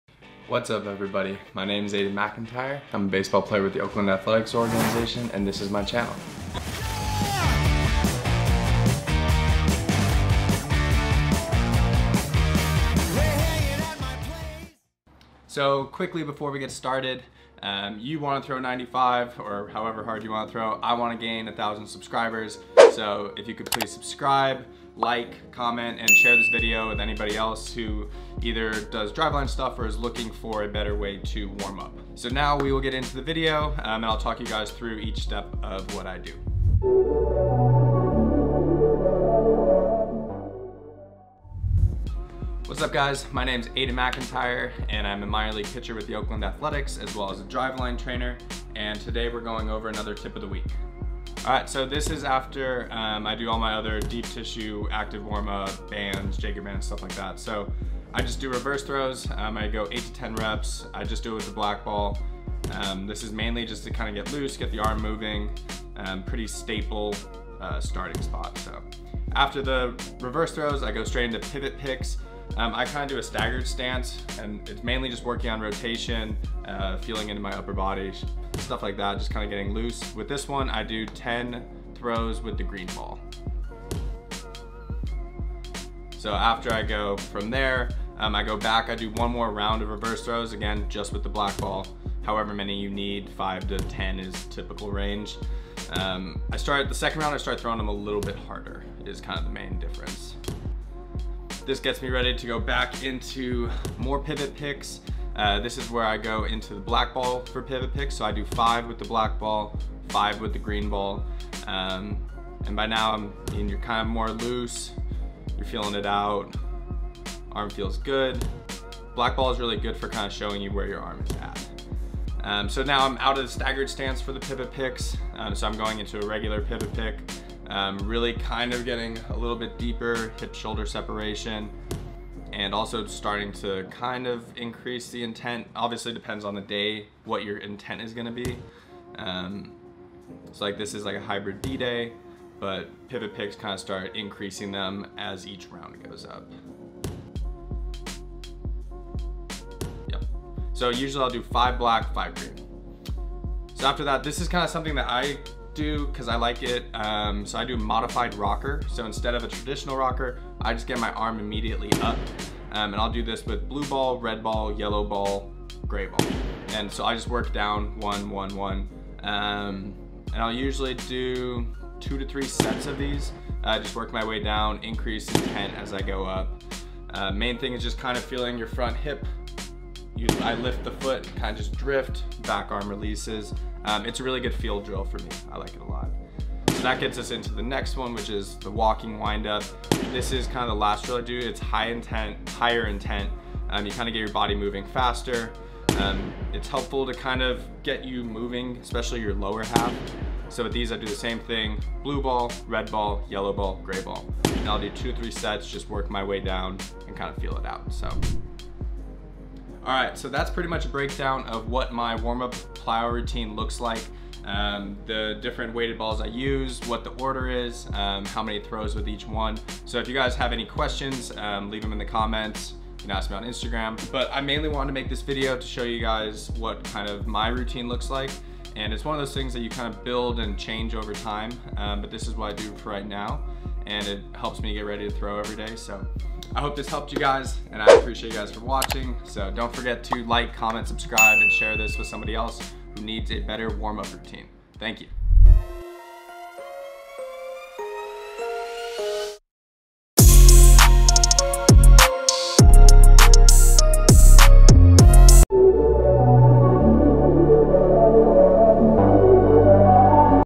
TIP OF THE WEEK | Breaking Down My Plyo Routine
Be sure to visit Aiden’s Instagram...
) to vote on future Tip of the Week topics! // The people are always asking about what a minor league player does with weighted balls, so here it is: Aiden McIntyre’s Plyo Ball Routine. Aiden goes over the drills he uses to warm up before throwing to keep his body safe and strong. This video features what plyo balls he uses, reps and sets. If you want to vote on the next Tip of the Week, remember to visit Aiden’s IG!